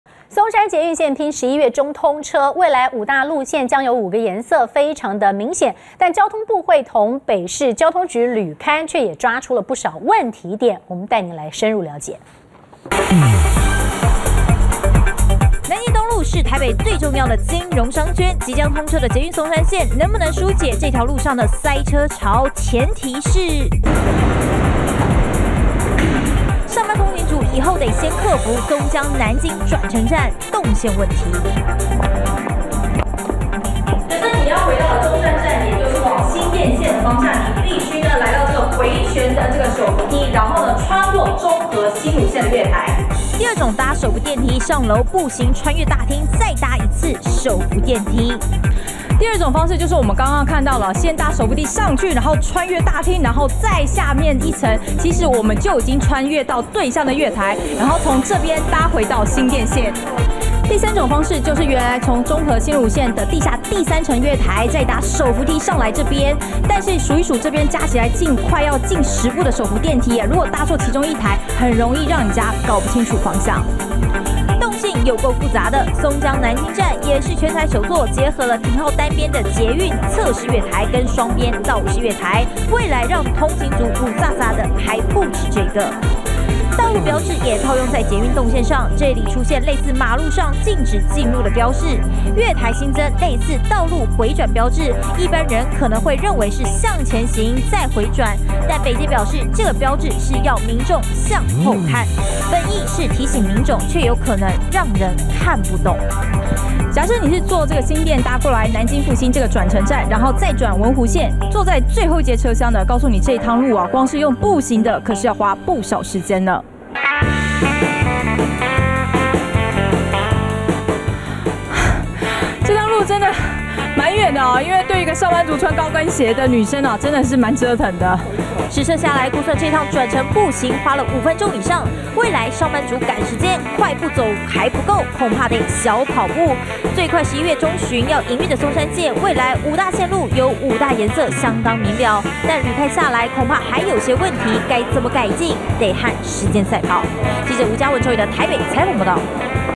20141101中天新聞 動線遠、標誌看嘸 松山線轉乘站待改進
松山捷運線拚11月中旬通車，未來5大路線會有5個顏色，相當明瞭。不過交通部會同北市交通局履勘，卻也抓出不少問題點，擔負重責大任的松山線貫穿南京東路，要替台北最重要的金融圈紓解上下班塞車潮，兩大轉乘站的南京復興跟松江南京，一個動線複雜、一個轉乘路線太遠，該怎麼改進？帶您搶先來看即將開通的松山線。